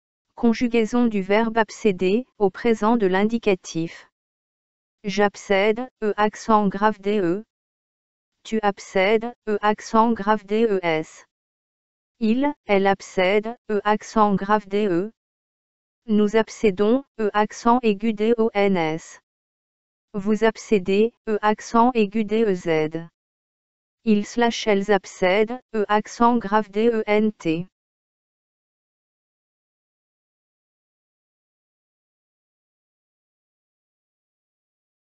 Abcéder | Présent de l'indicatif
Conjugaison du verbe Abcéder, au présent de l'indicatif.
j'abcède
tu abcèdes
il/elle abcède
nous abcédons
vous abcédez
ils/elles abcèdent